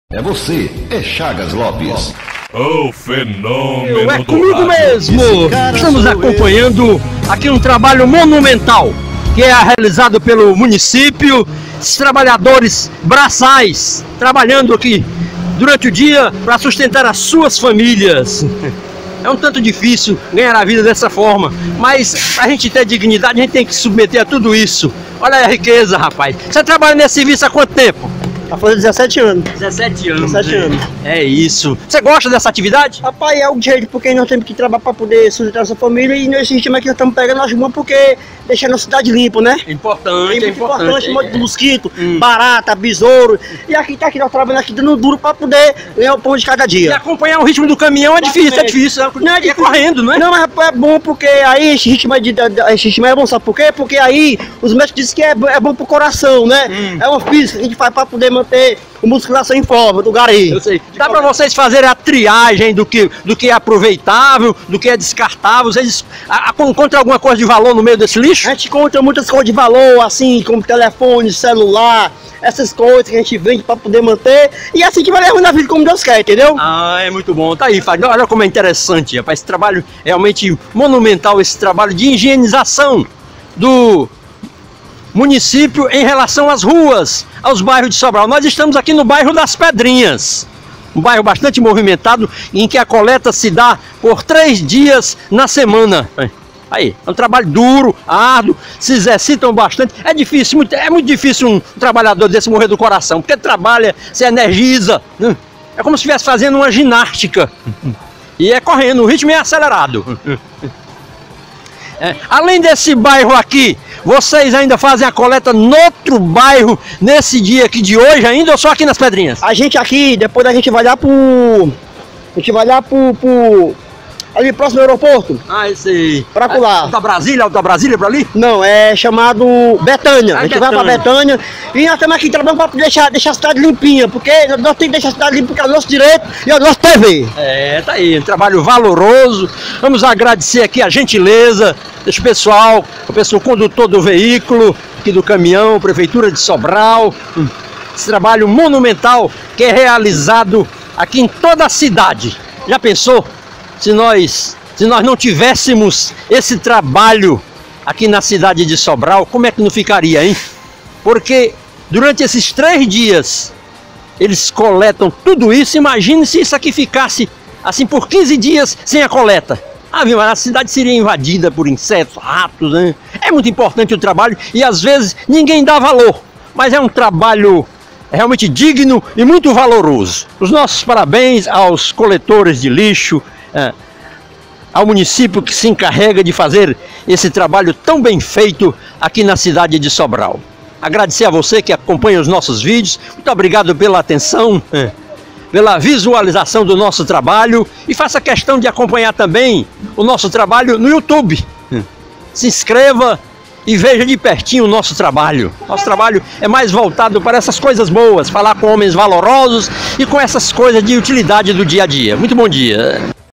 Radialista Chagas Lopes com os coletores de limpeza da cidade
Acompanhamos de pertinho a coleta de lixo, aqui mesmo na cidade de Sobral-Ce, e comprovamos o quanto é importante o trabalho destes coletores, como também para a limpeza da nossa cidade.💻🎤🎥